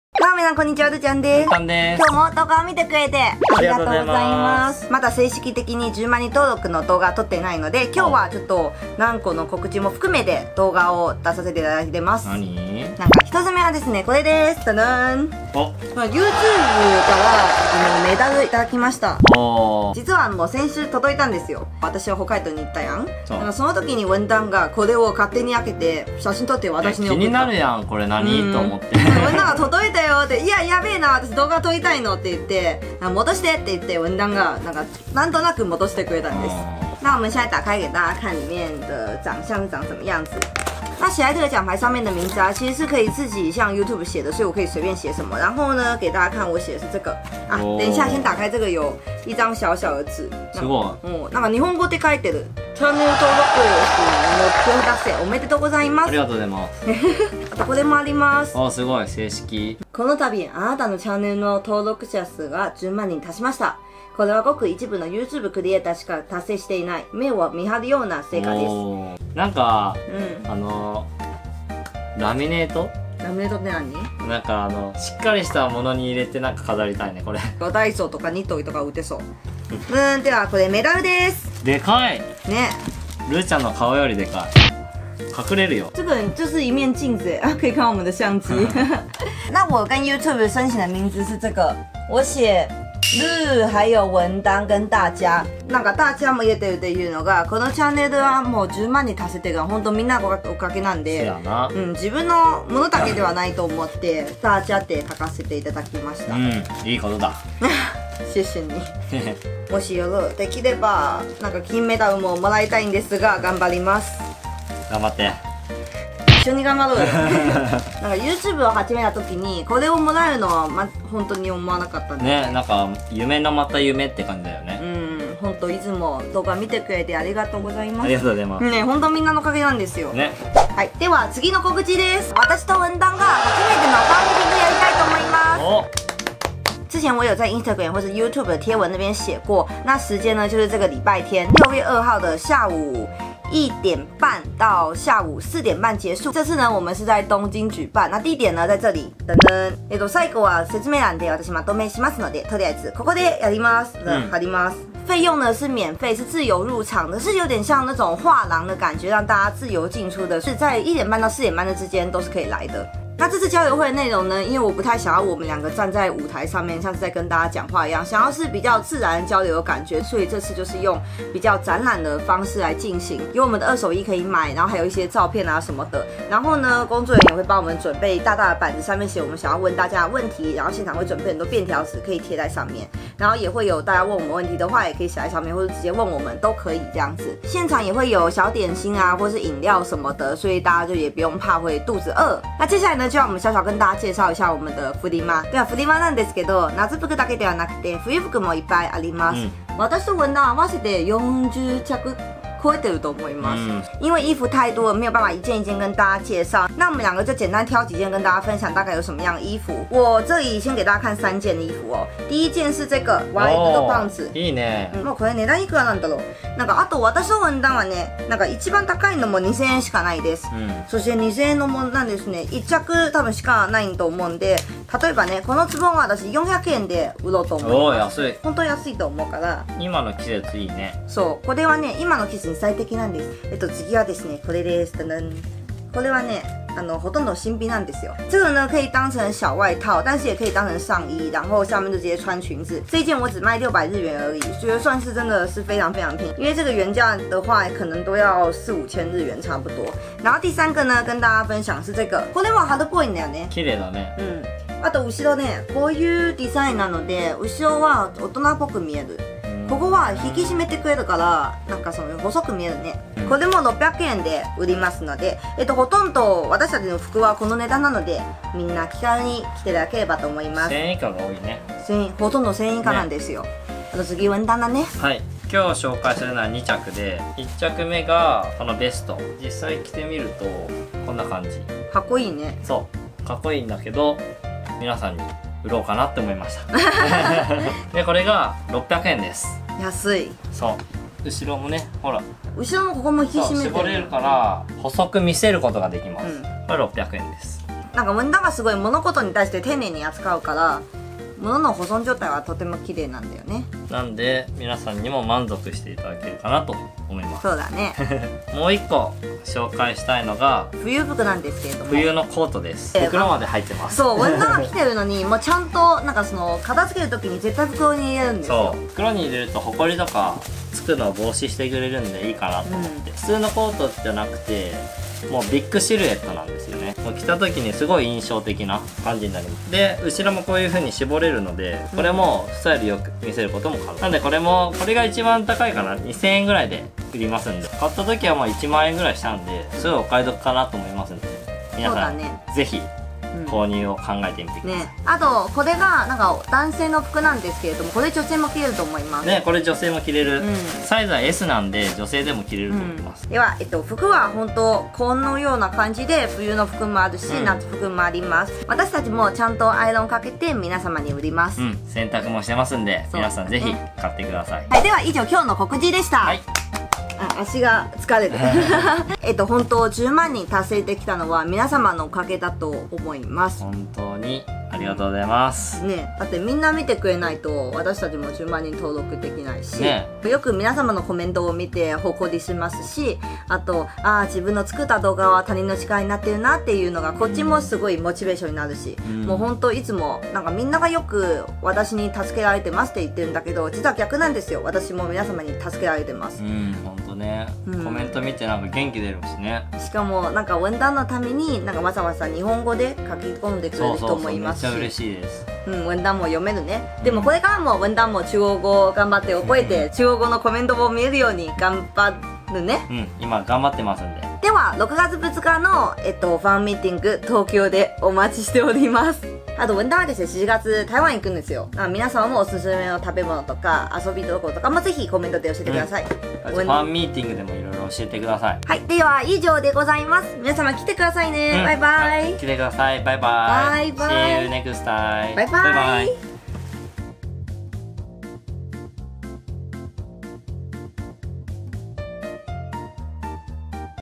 終於要辦了...😭10萬訂閱見面交流、二手衣拍賣會！期待跟大家見面😋❤️｜RU
十萬訂閱紀念見面交流會

日期：6/2 13:30 START（預計於16:30結束）
場地：GOODOFFICE六本木 〒106-0032 東京都港区六本木7-3-24 A-1
費用：免費，自由入場
內容：紀念10萬訂閱的見面交流會！有二手衣市場、留言板、我的軌跡展示。當日會準備小點心和飲料，期待大家來玩～

重要！
要買二手衣的大家記得響應環保自備購物袋唷～！